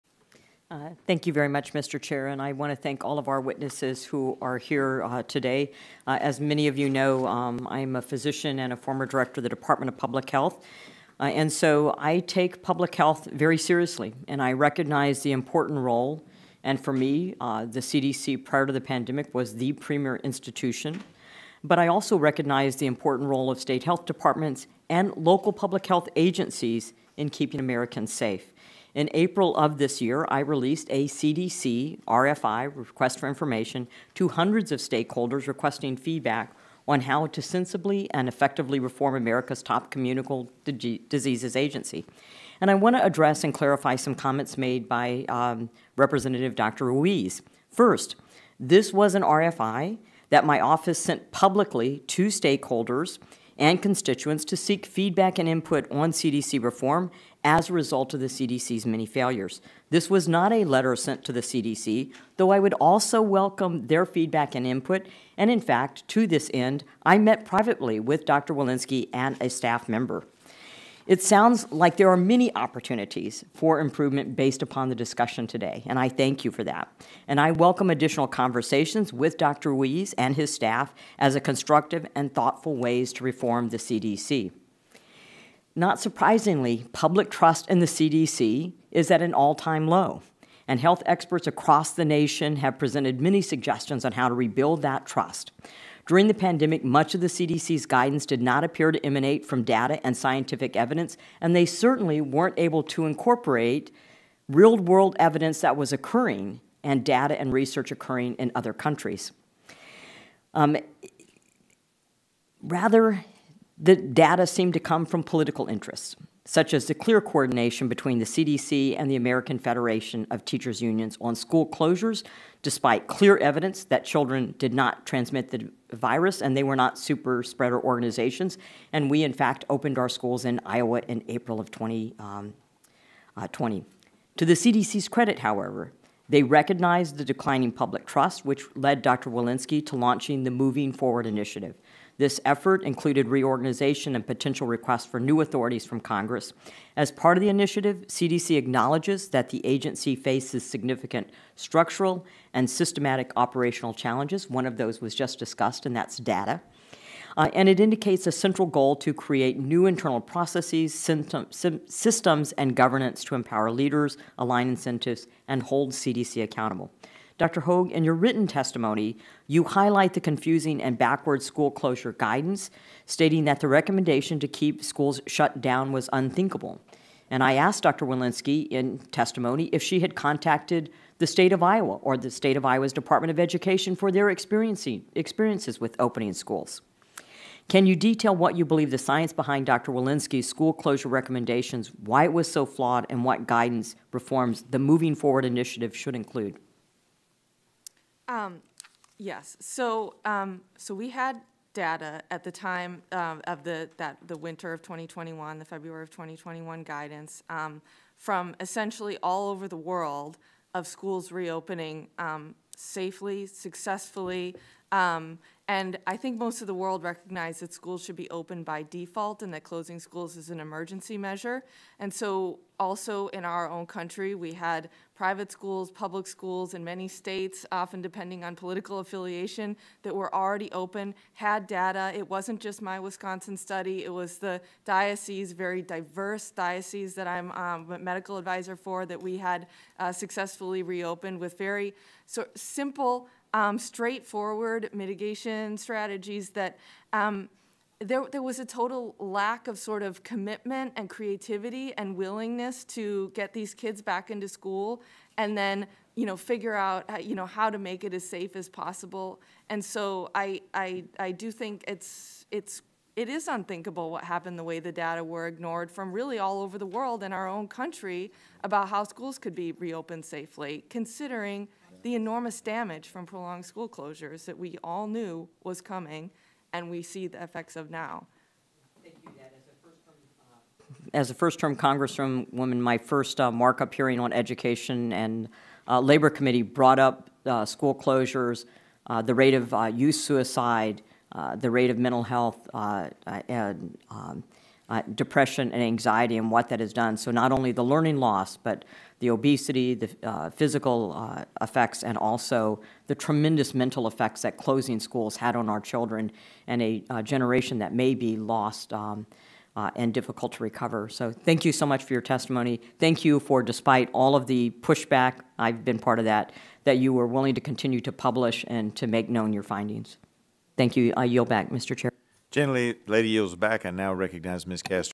'Public Trust In The CDC Is At An All-Time Low': Mariannette Miller-Meeks Urges Health Policy Shifts
Rep. Mariannette Miller-Meeks (R-IA) questioned witnesses about school reopening procedures during the pandemic at a House Energy Committee hearing earlier this month.